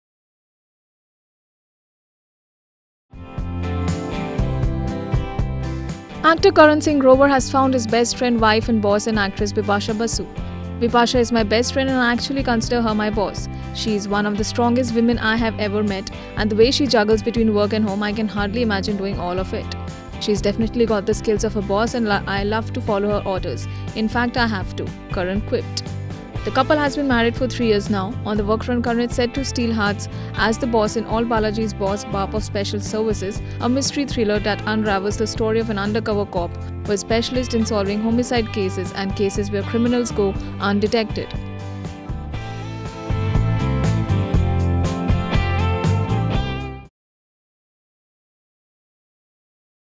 Karan Singh Grover considers wife Bipasha his boss
Actor Karan Singh Grover has found his best friend, wife and boss in actress Bipasha Basu.